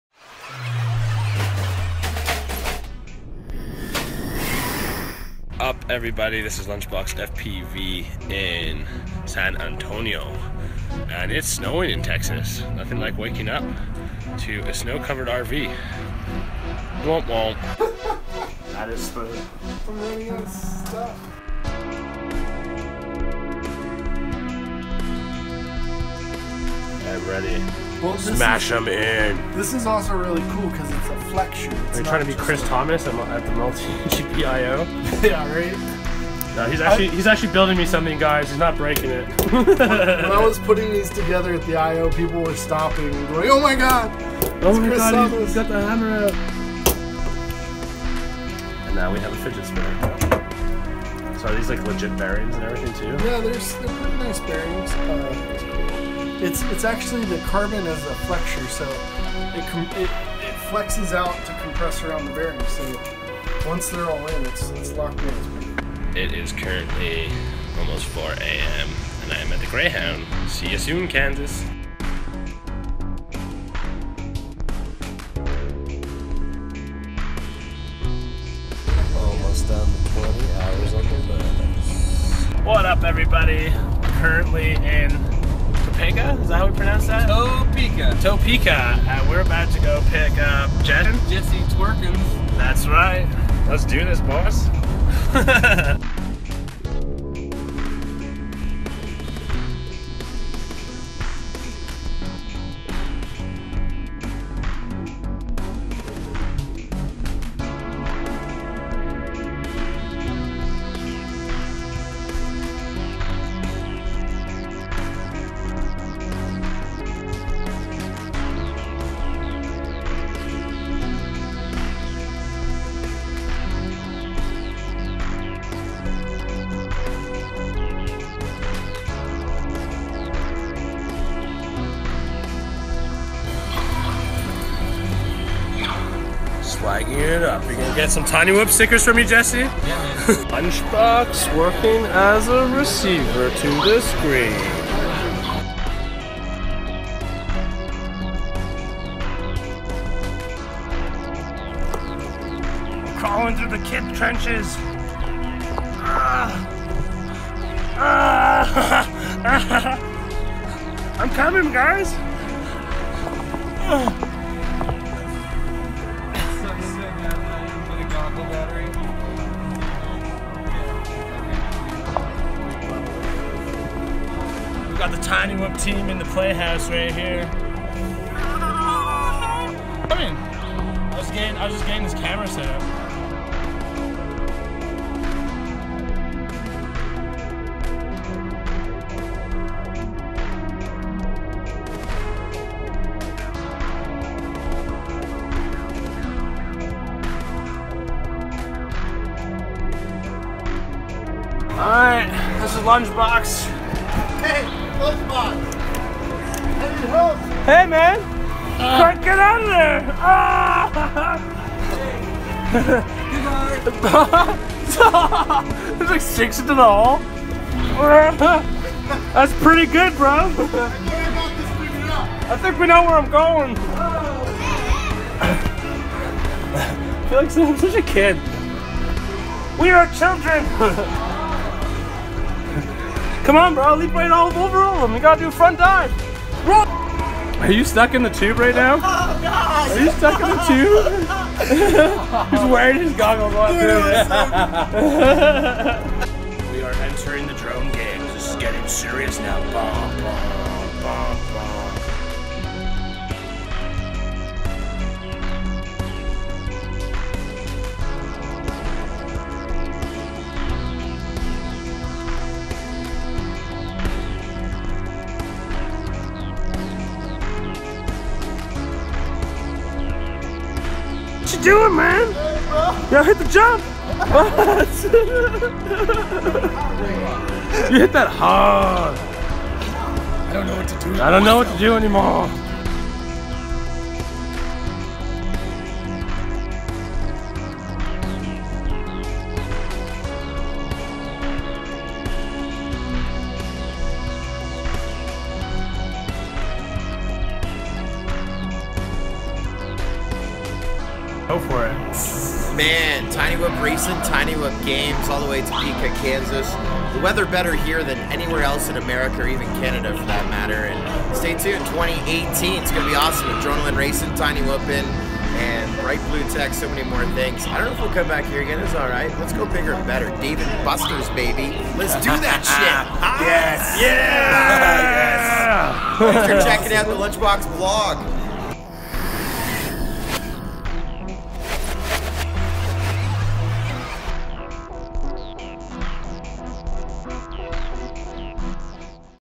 Lunchbox On Wheels - Day 30 - THE END
Quick stop in Texas to meet up with Josh from Flynoceros frames (link below), what an awesome person and great passion for the industry! Then I got on a bus for what felt like forever LOL  But I made it to Kansas for my LAST STOP in Topeka for the aDRONEaline Tiny Whoop race... I came 1ST!!! Truly felt like I was leaving family, Jesse, Cody, Austin, Joe Scully, what a way to end my trip with you famous faces :P LUNCHBOX OUT ... for now ;)

***Huge thank you again to all the people who supported me throughout this trip. I love this community and I am so happy I gave up everything up in my life for FPV.